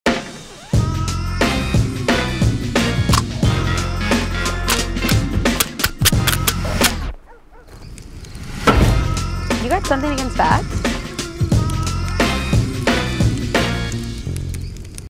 Josh Hader Breaks Bats with Solo Stove
Superstar Relief Pitcher Josh Hader REALLY hates bats.